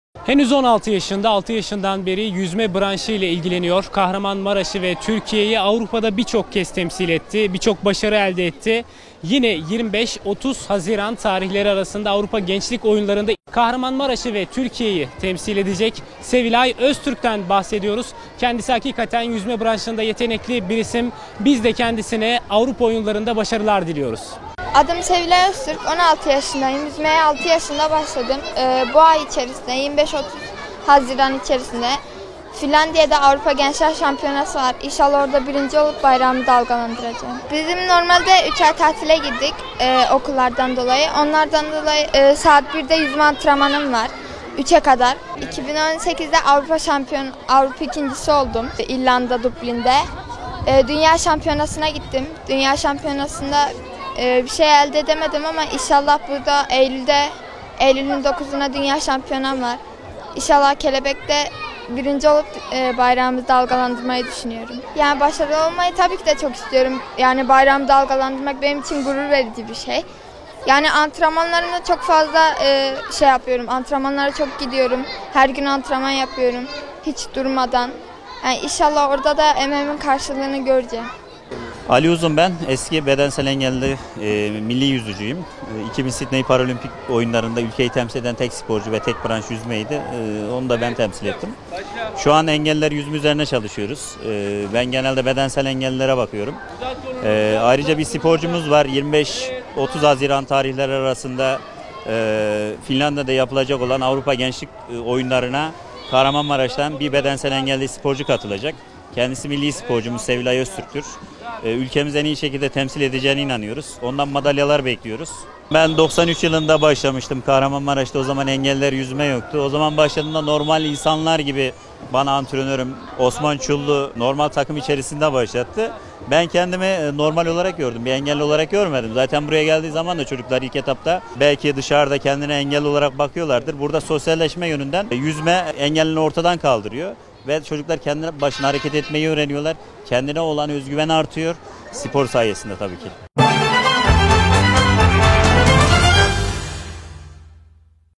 SEVİLAY ÖZTÜRK AVRUPA'DA TÜRKİYE'Yİ TEMSİL EDECEK
Kahramanmaraşlı 16 yaşındaki Sevilay Öztürk, Türkiye'yi Finlandiya'da düzenlenecek olan Avrupa Gençlik Oyunları'nda temsil edecek.
Milli Yüzücü'nün hedefi ise Avrupa Şampiyonu olmak.